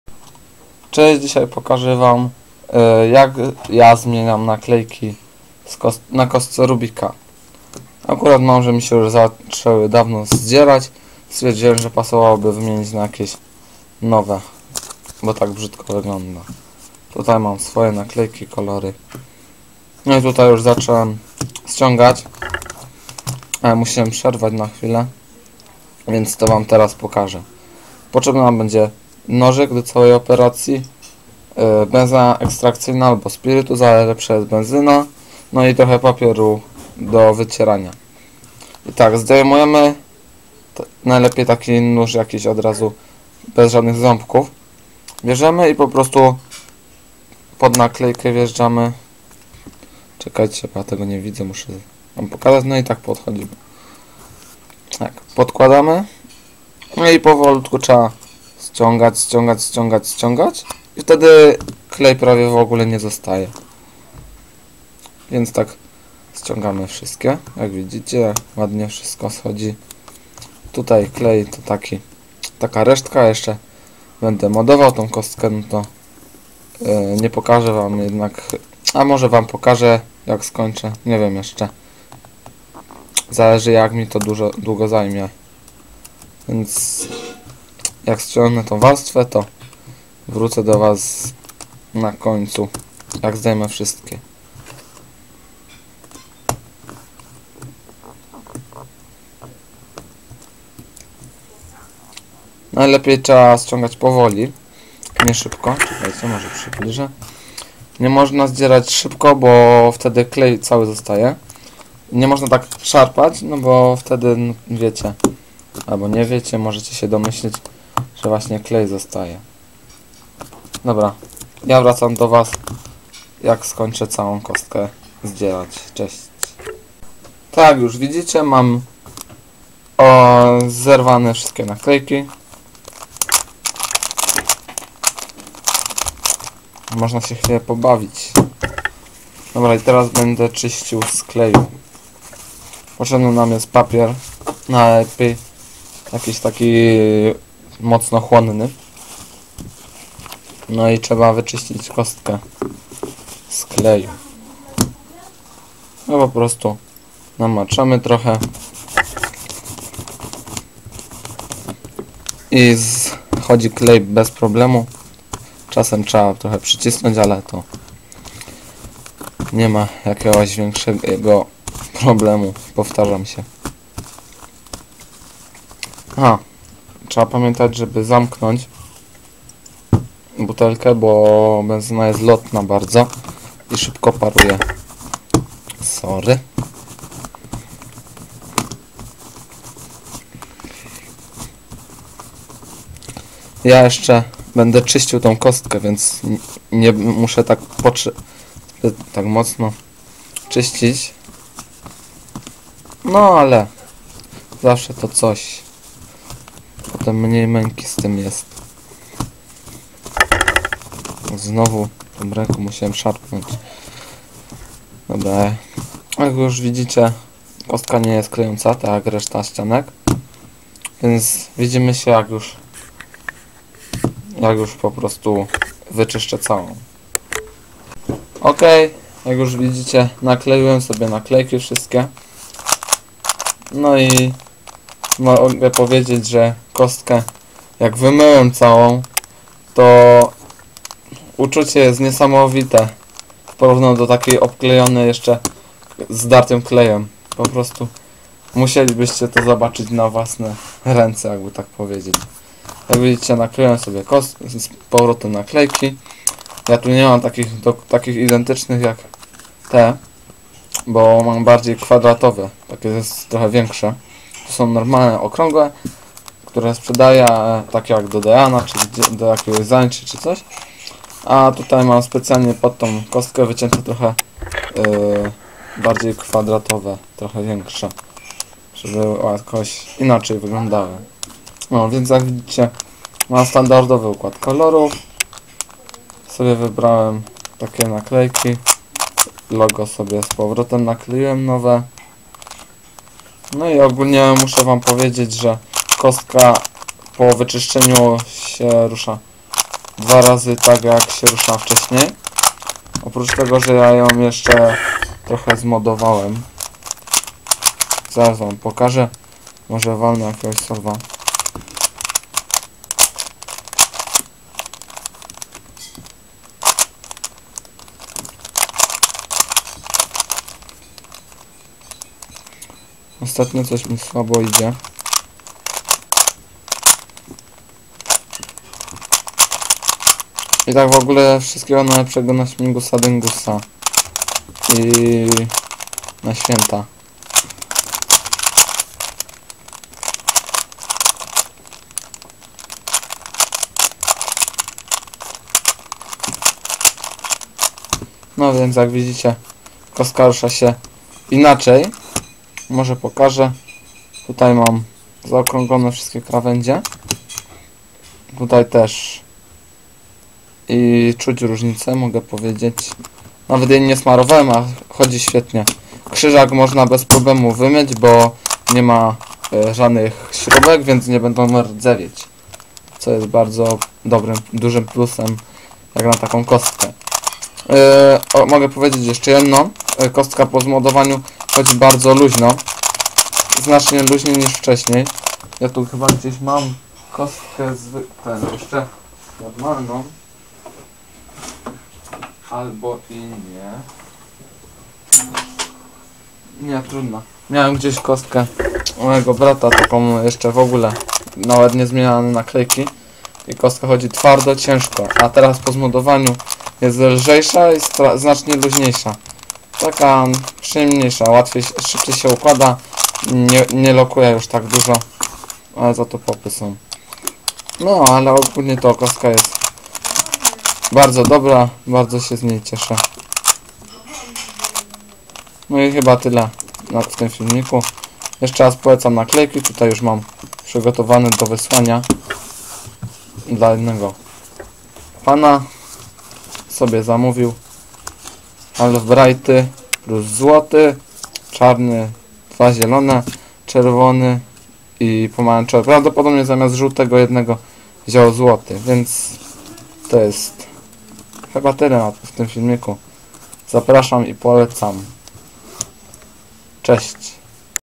Zmiana Naklejek Na Kostce Rubika
taka tam zmiana naklejek na nowe